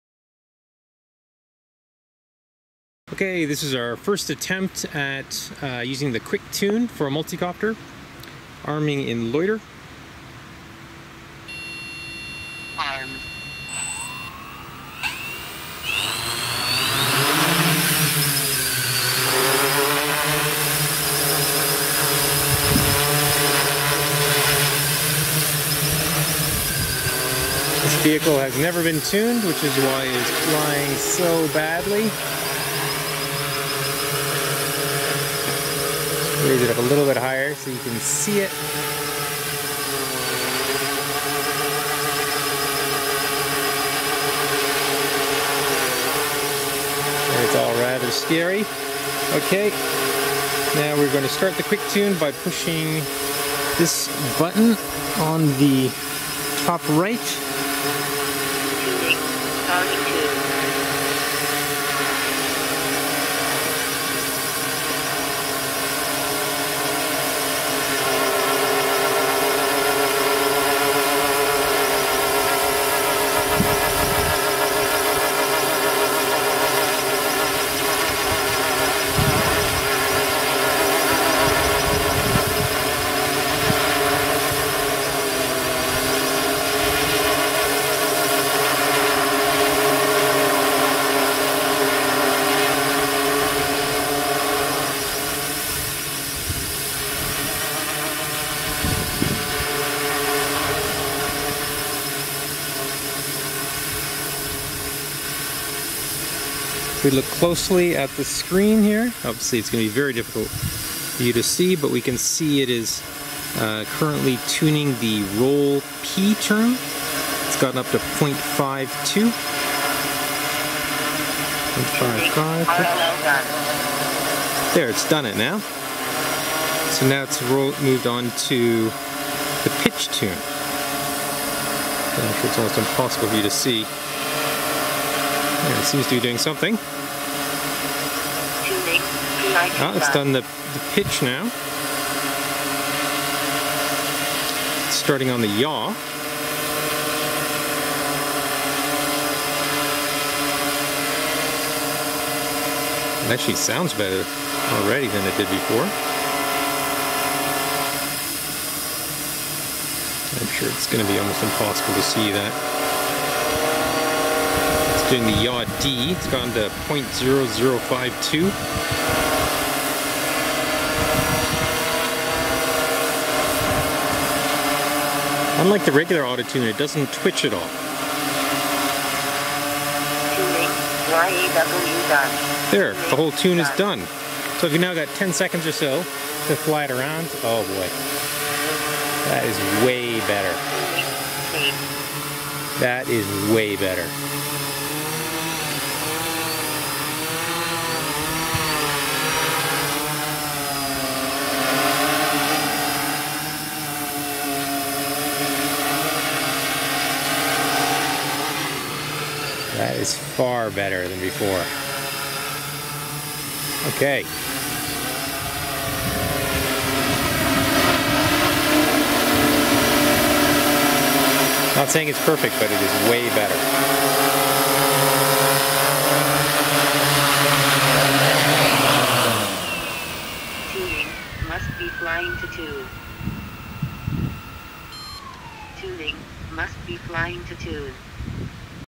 VTOL Quick Tune First Test (on a multicopter)
This is a first attempt at using ArduPilot 4.3.0-DEV with the VTOL Quick Tune Lua script which was originally designed for QuadPlanes but also works for multicopters.
Frame: Hexsoon TD650 with CubePilot CubeOrange running ArduPilot Copter-4.3.0-DEV